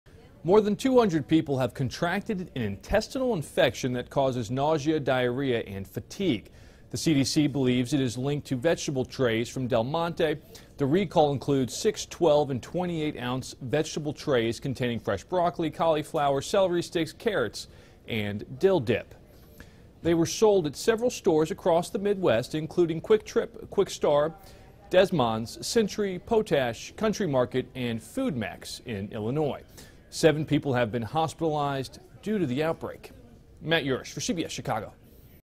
FDA: Parasites In Del Monte Vegetables Causing 'Explosive' Diarrhea
Over 200 people have now been infected by an outbreak of parasites linked to recalled vegetable trays from Del Monte. Matt Yurus reports.